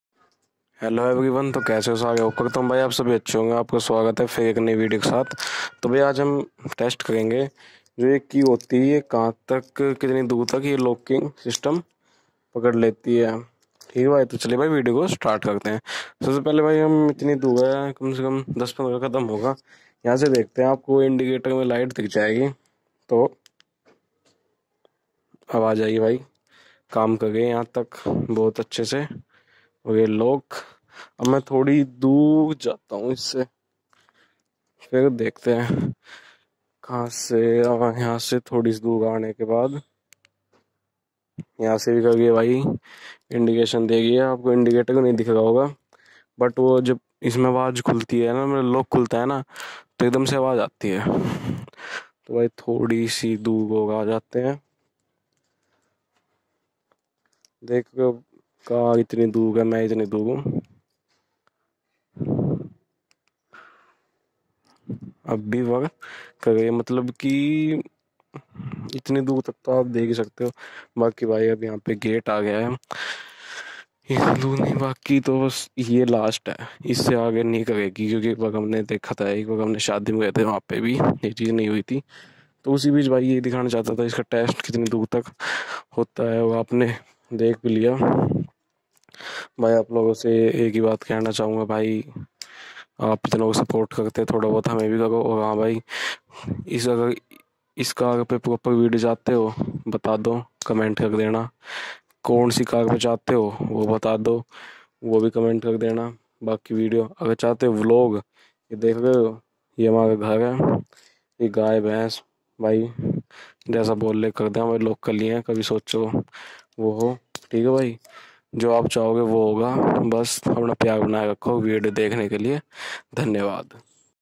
car central locking ultimate range test 2020 | shocking result
car central locking ultimate range on not only this car all car because technology day by day smart.

video owner-labbi rao from Gurgaon Haryana HR26.